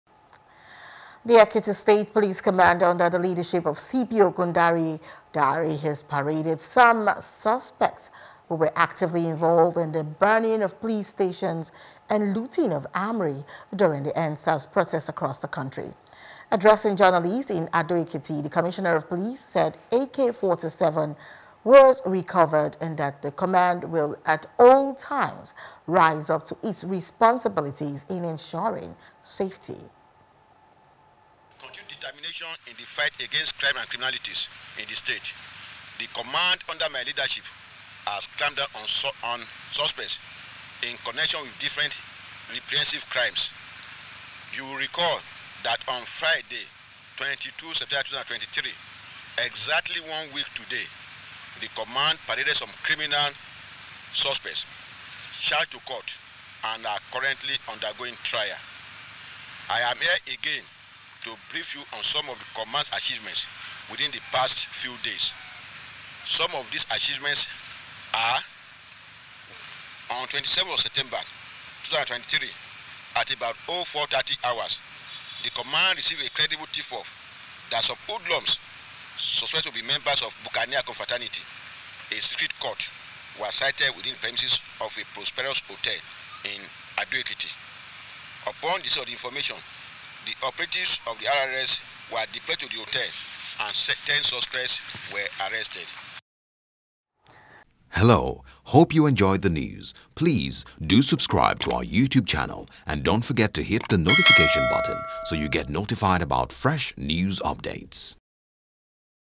Cultism: Police Parade Suspected Cultist In Ekiti
The Ekiti state police command under the leadership of CP Ogundare Dare has paraded some suspects who were actively involved in the burning of police stations and looting of armoury during the endsars protest across the country.